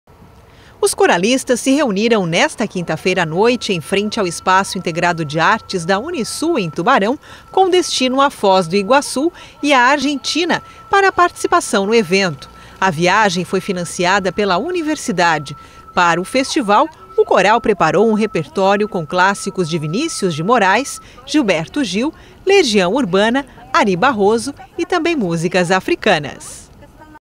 Coral Unisul participa de festival internacional
O Coral Unisul completa em 2015 vinte anos e vai participar de vários eventos envolvendo o canto coral. O primeiro deles, será o quarto festival Internacional Três Fronteiras, neste fim de semana, em Foz do Iguaçu